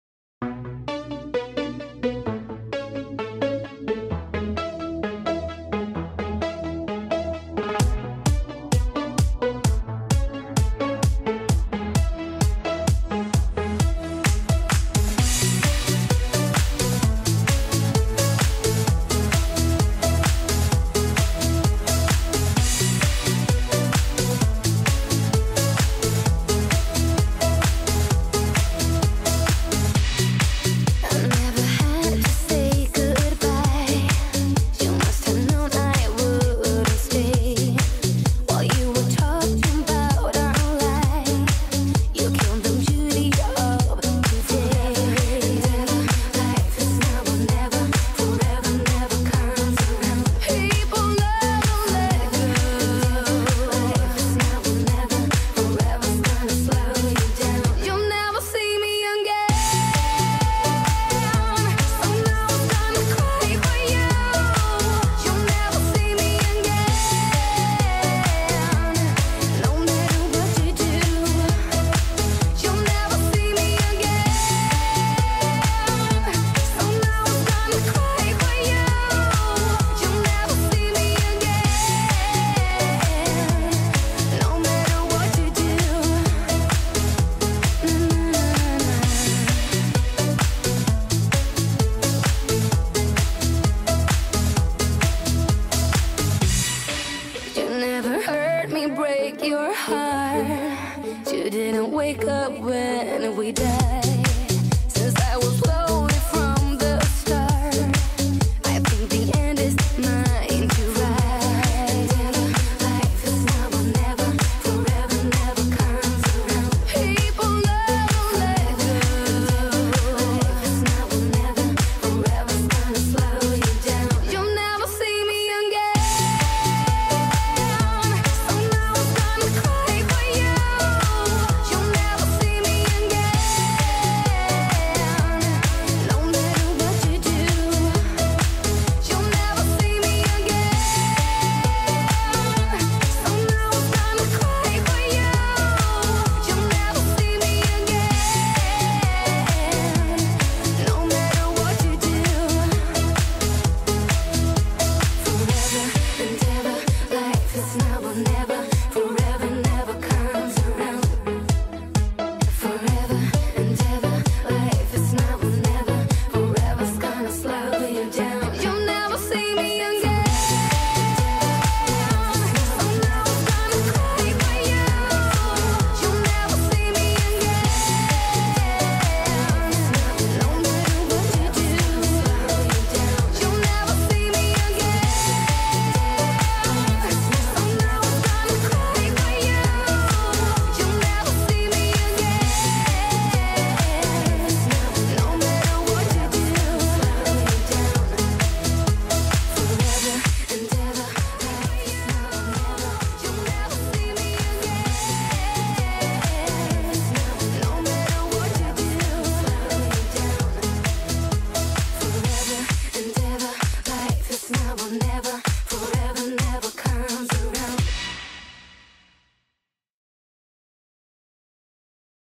1° Concurso Model Capoo's Perfil
Adrianapaula Capool e Aschdley McMillan a frente do mais belo desfile do SL